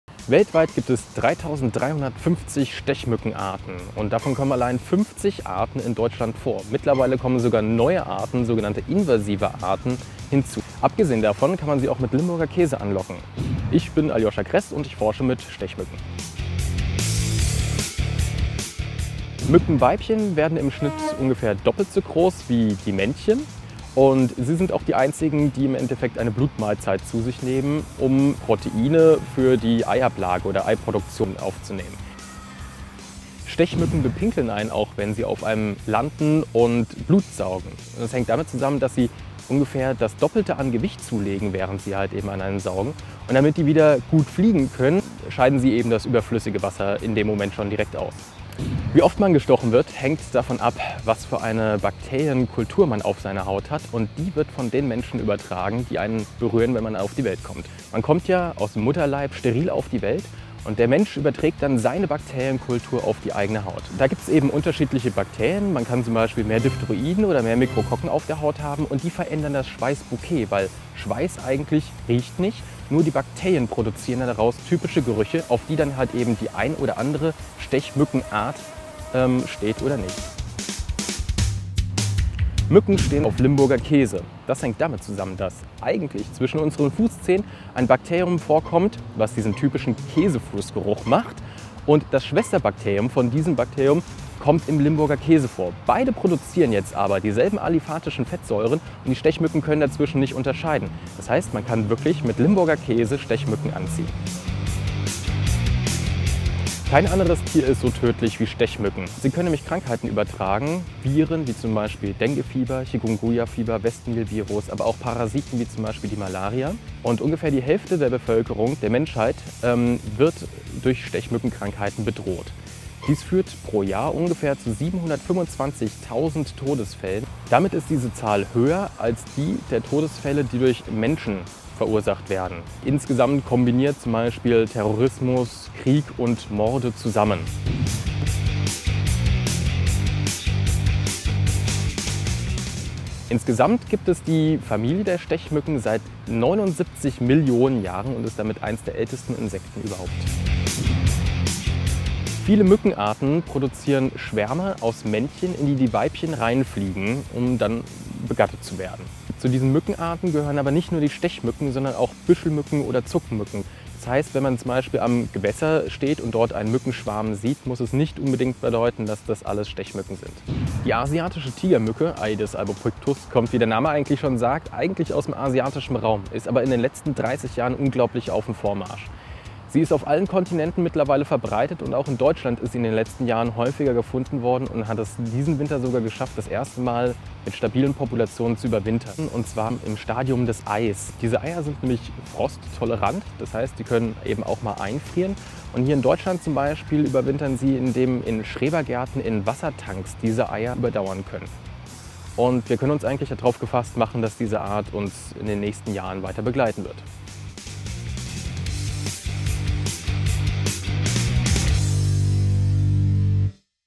Mücken: Darum stechen sie manche Menschen häufiger und 7 andere spannende Fakten über die Blutsauger
Mücken sind die tödlichsten Tiere der Welt. Die Anzahl der Menschen, die an von Mücken übertragenen Krankheiten sterben, ist höher als die Zahl der menschlichen Todesfälle, die vom Menschen verursacht werden. Diese und weitere spannende Tatsachen über die Blutsauger erzählt Euch Biologe Aljoscha Kreß. Er hat in seiner Doktorarbeit am Senckenberg Forschungsinstitut in Frankfurt die asiatische Tigermücke erforscht.

Bildquellen:

Titel
Aedes aegypti mosquito
James Gathany, 2006, CDC

Fakt 1
Via Pixabay:

Fakt 2
Aedes aegypti mosquito

Fakt 4
Limburger Käse
Markus Hagenlocher, 2006
Fakt 5
Culex larvae
James Gathany, 2003, CDC

Fakt 6:
Mücke auf Moos via Pixabay

Fakt 7:
Mükenschwarm via Pixaby

Aedes albopictus
James Gathany, 2002, CDC

Fakt 8:
Aedes albopictus - eggs - Aedes albopictus
Jeffrey Tucker, Houston, Texas, USA

Regentonne
S.J. de Waard, 2010